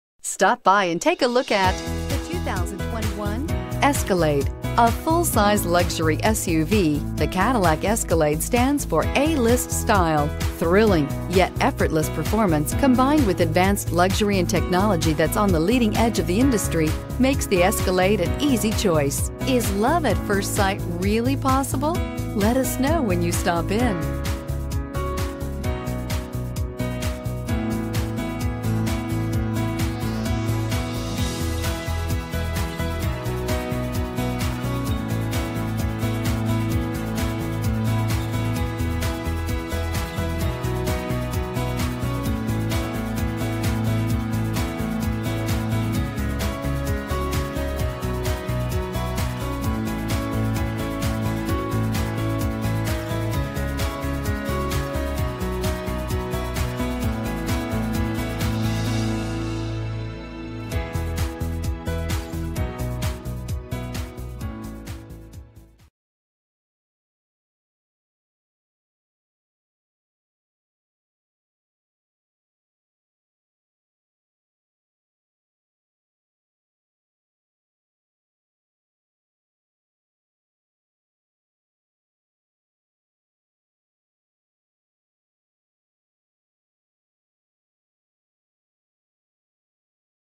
2021 Cadillac Escalade Garland TX MR287062
2021 Cadillac Escalade Sport Platinum

Garland Cadillac
11675 LBJ freeway

Shadow Metallic 2021 Cadillac Escalade Sport 4WD 10-Speed Automatic with Overdrive 6.2L V8 4WD, Whisper Beige With Gideon Accents Leather.Recent Arrival!Awards:* ALG Residual Value